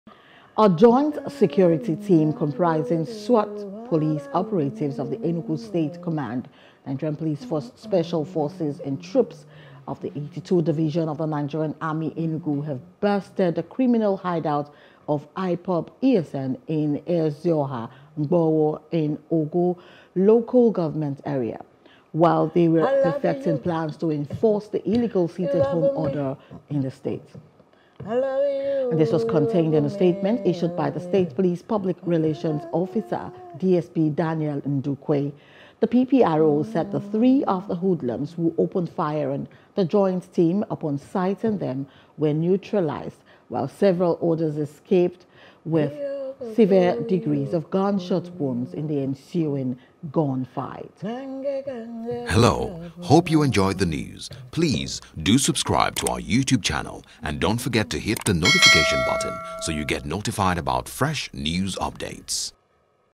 Enugu: Security Neutralise 3 IPOB/ESN Hoodlums In Gunfight, Recover Weapons
A joint security team, comprising SWAT Police Operatives of the Enugu State Command, NPF Special Forces, and Troops of the 82 Division of the Nigerian Army Enugu have busted the criminal hideout of IPOB/ESN in Ezioha Mgbowo in Awgu Local Government Area, while they were perfecting plans to enforce the illegal sit-at-home order in the State.